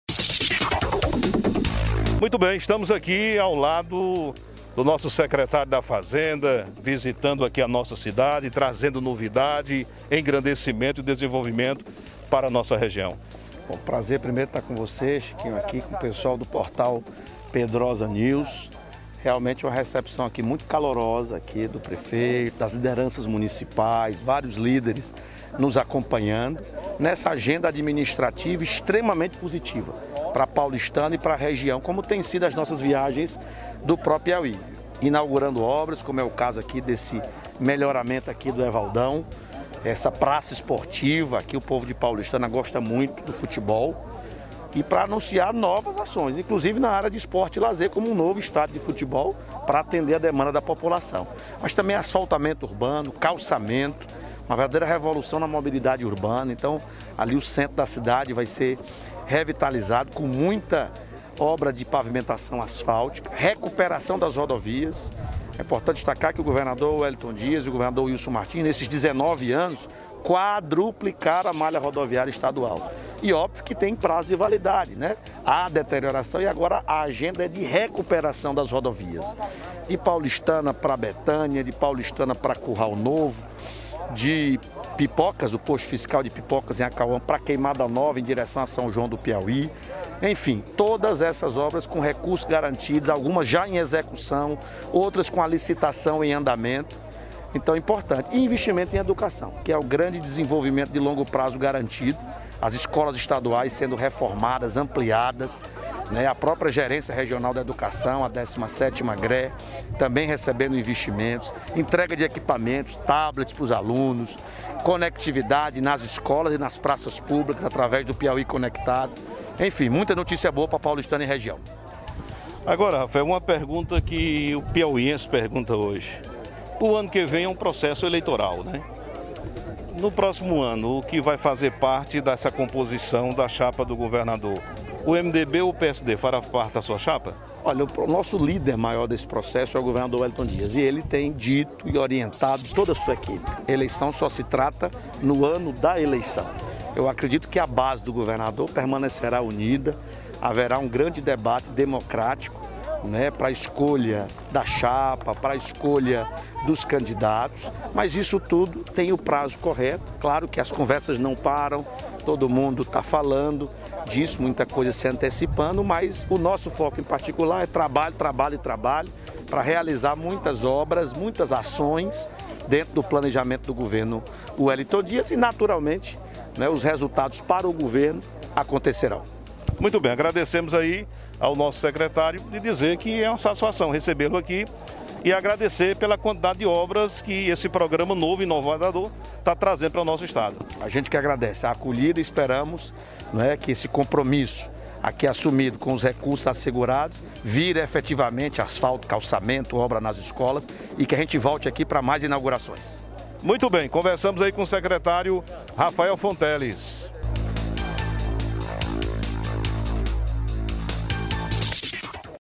RAFAEL FONTELES FALA SOBRE OBRAS E INVESTIMENTOS DO PRO PIAUÍ EM PAULISTANA
O secretário de Fazenda e coordenador do Pro-Piauí, Rafael Fonteles visitou Paulistana nesta quarta-feira, 6, e falou sobre obras e investimentos no município.